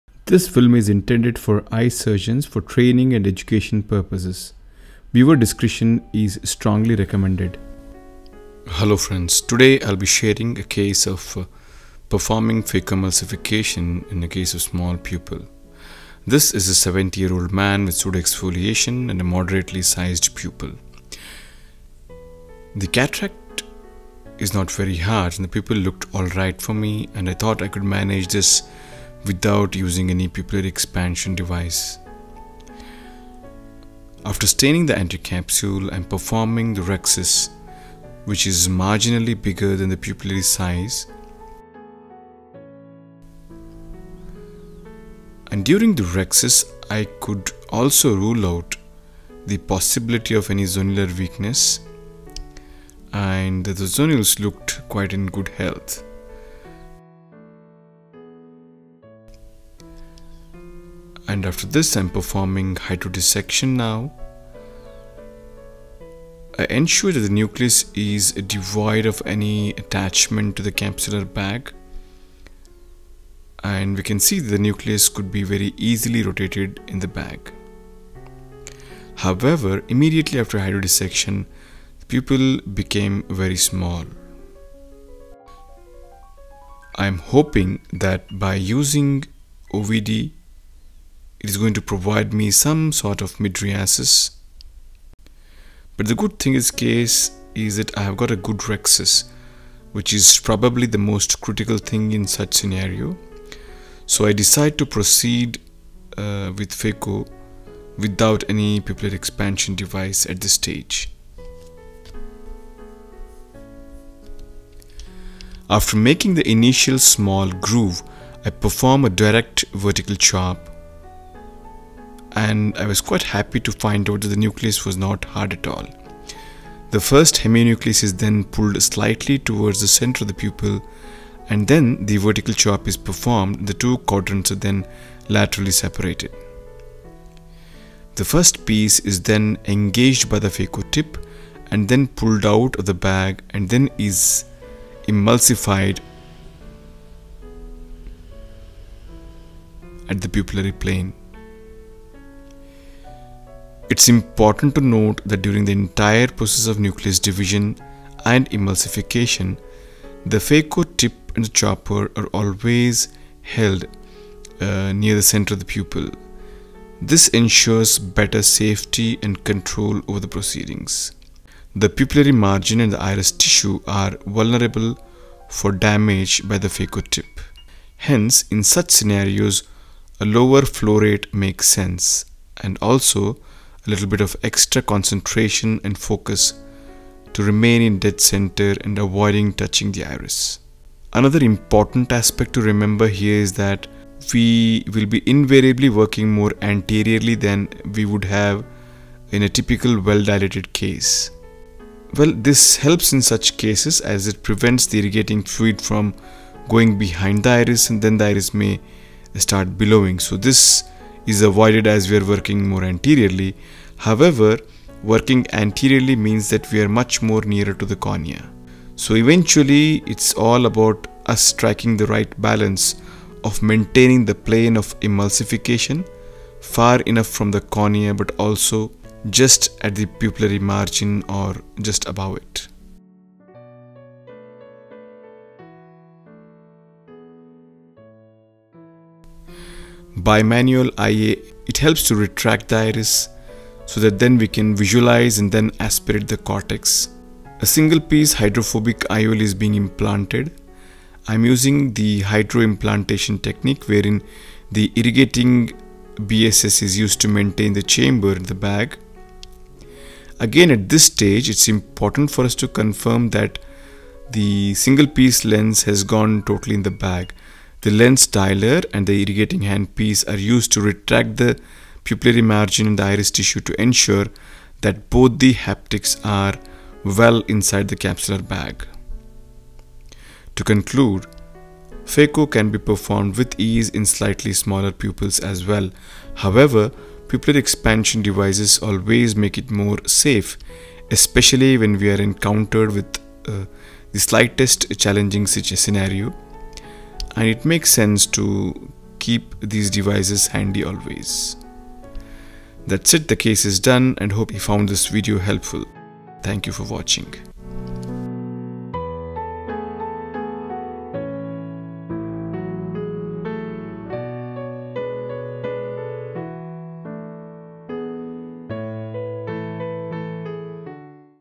Phacoemulsification in an eye with small pupil - Understanding strategies , Dr Deepak Megur
This video highlights certain surgical strategies while performing phaco emulsification in an eye with pseudoexfoliation and small pupil.
In this case no pupillary expansion devices has been used. What precations need ot be taken in such a scenario..?

Below there are links to my other films on small pupil surgery